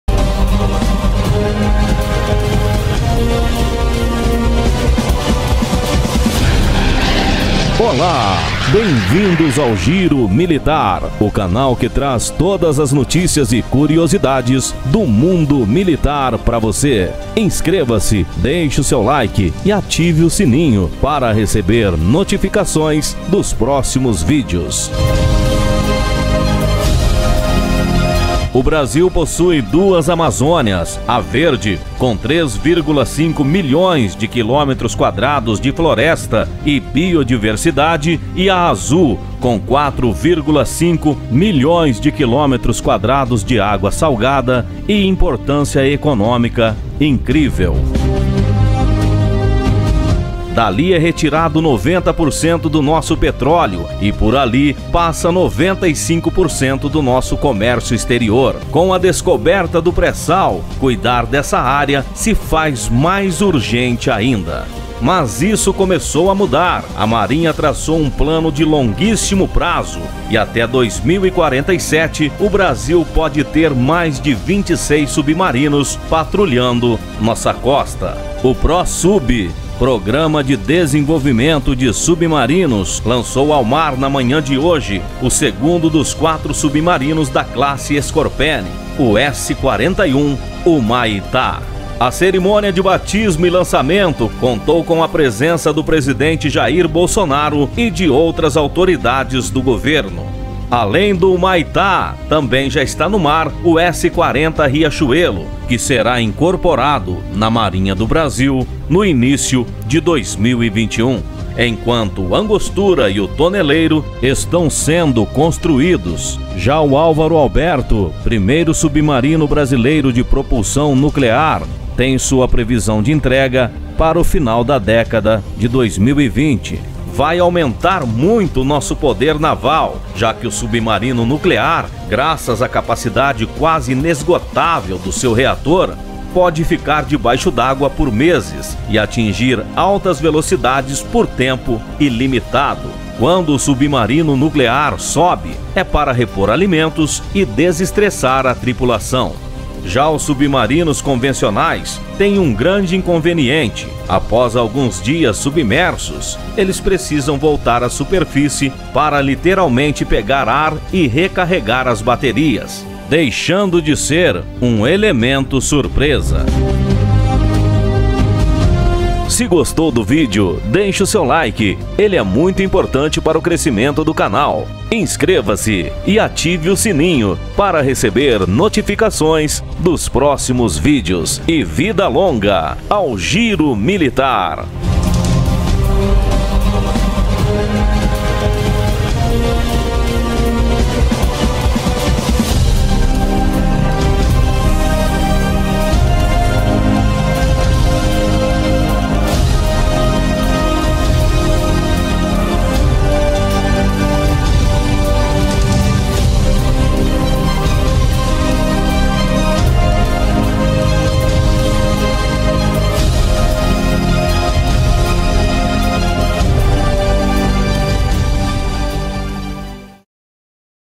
Submarino Humaitá foi batizado! Exército Brasileiro, Marinha do Brasil e FAB
Exército Brasileiro, Marinha do Brasil e Aeronáutica! Cada vez mais forte!

O PROSUB, Programa de Submarinos Brasileiro  lançou ao mar na manhã de hoje, o segundo dos 4 Submarinos da classe Scorpene, o S41 Humaitá.